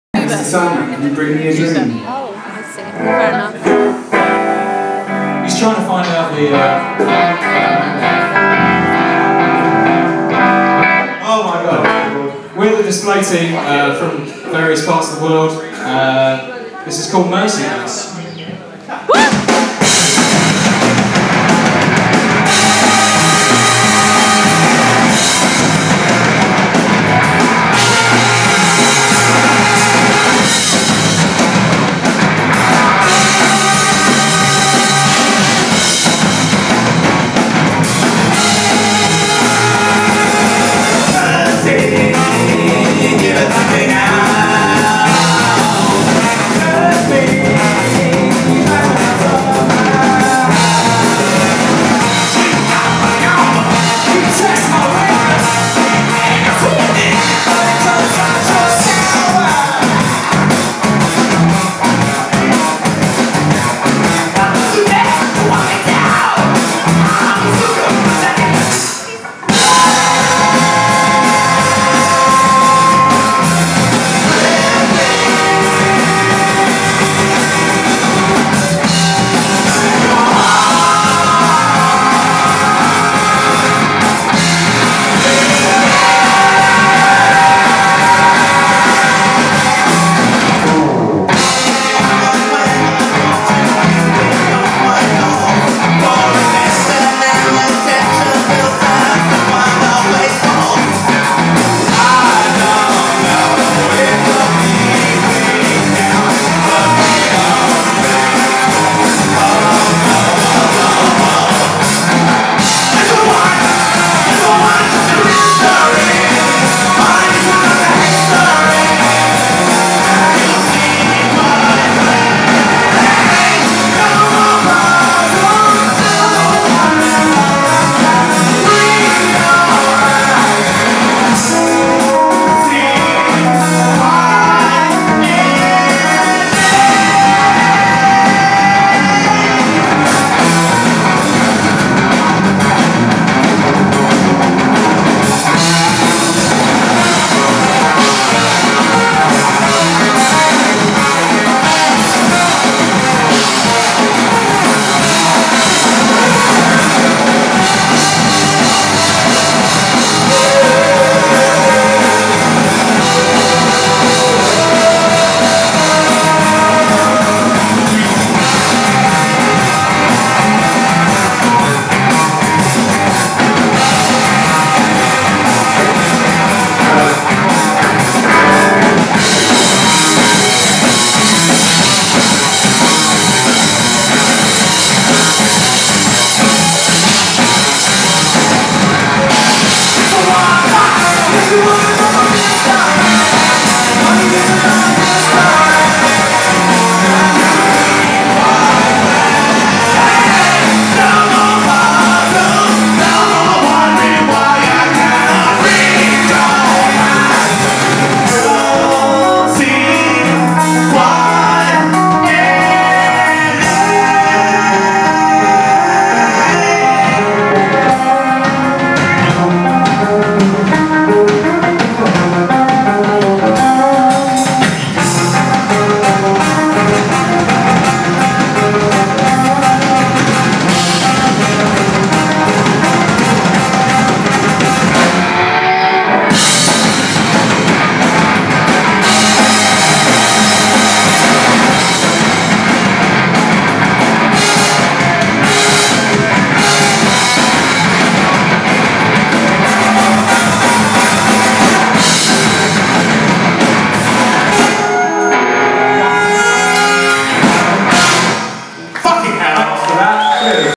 display team @ Nutshell Live! (16 Oct 2011)
Display Team play at Nutshell Live! - Old Queen's Head, London